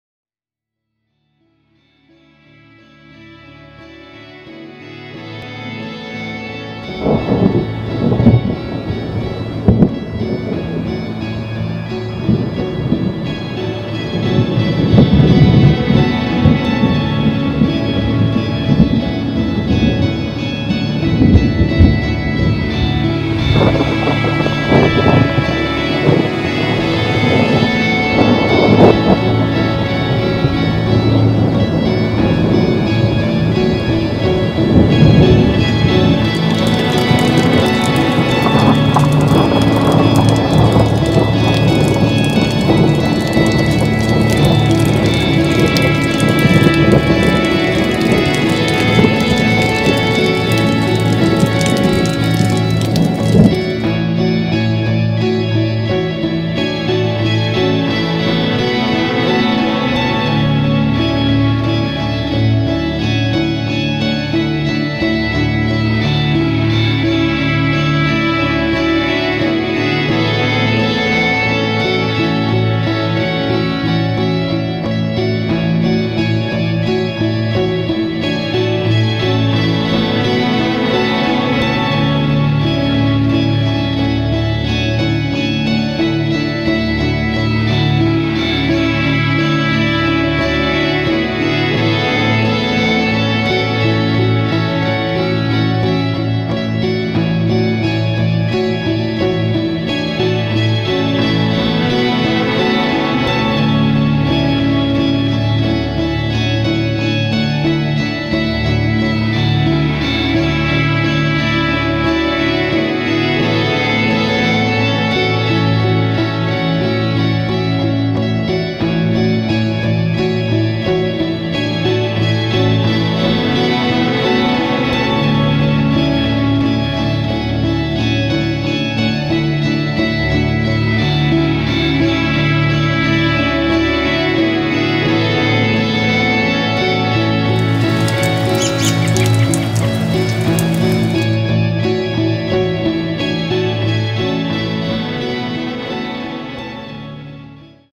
Thunderstorm, Hailey, Idaho 05.07.2016.
Some cool images of the beauty of a storm. In memory of my mom. Produced by Steve Ferguson, Sun Video Productions, FERGTV.COM Hailey, Idaho.